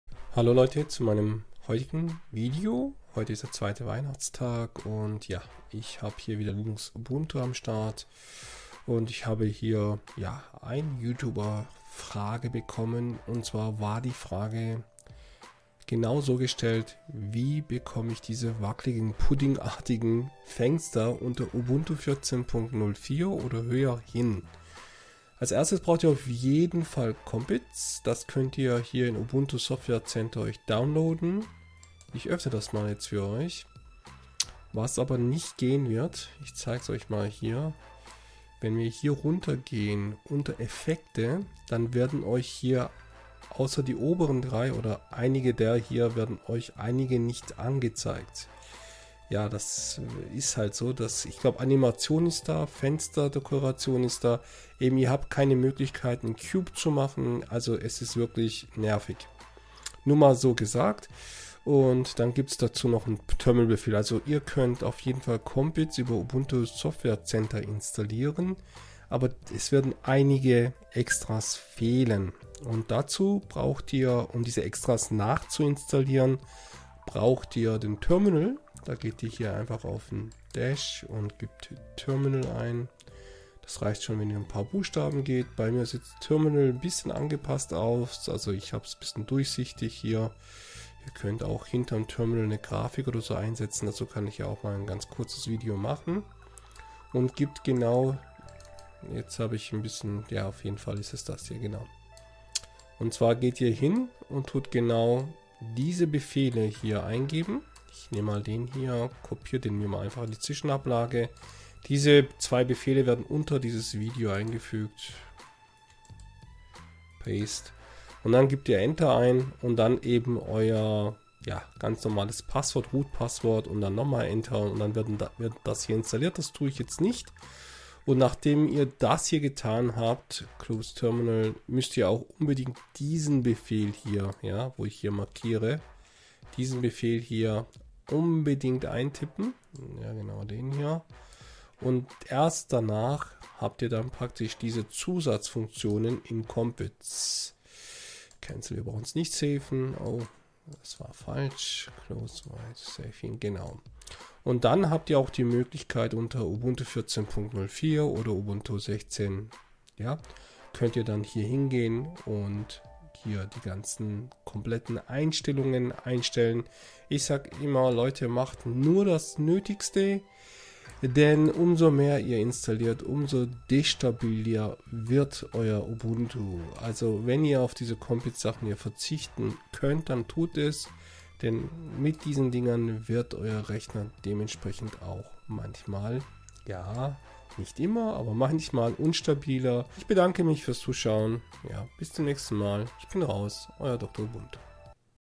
Wackelige Fenster unter Ubuntu 14.04
Wackelige Fenster einstellen unter Ubuntu. Wie geht das?
Terminal Befehle
[1] sudo apt-get install compizconfig-settings-manager
[2] sudo apt-get install compiz-plugins-extra